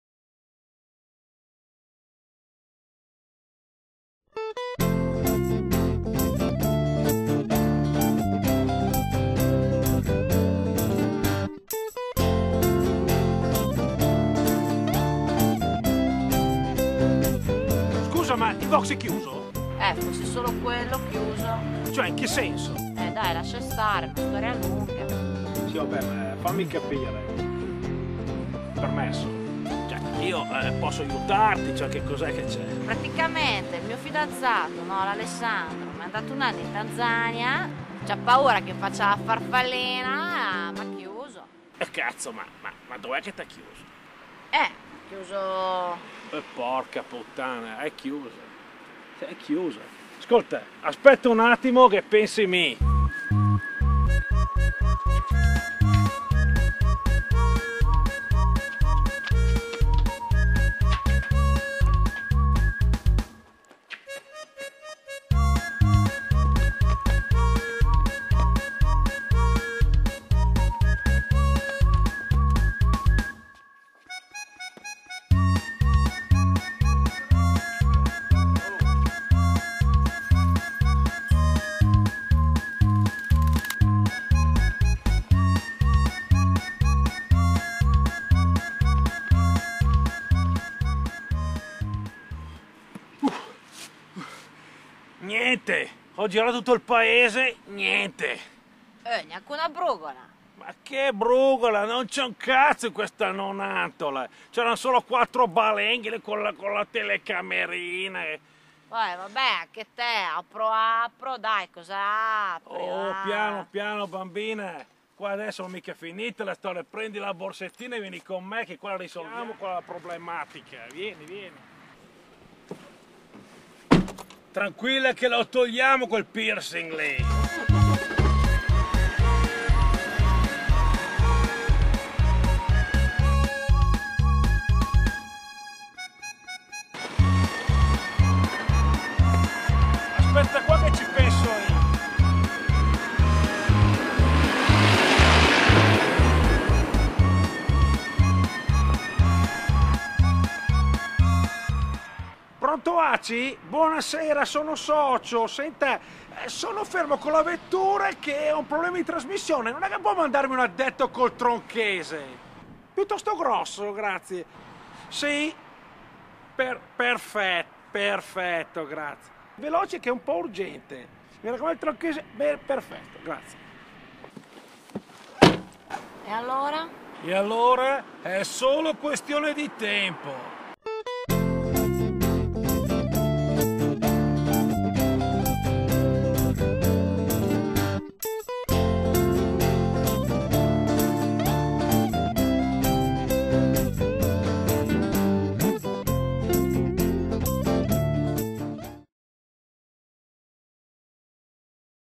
Nonantola città aperta
Vincitore miglior cortometraggio nonantola film festival 2008. Premio in palio: 1000 euro. Durata massima per la realizzazione: 4 giorni. Elementi obbligatori: un lucchetto, un giubbino catarifrangente, frase (è solo questione di tempo), doveva comparire nonantola almeno in una scena. Le musiche dovevano essere originali. Durata massima: 4 minuti. Genere sorteggiato: comico

soggetto e sceneggiatura:
Alessandro Scillitani
Fabio Paini
Alessia Musi
Mimmo Campanile
Gianni Binelli

regia e musiche:
Alessandro Scillitani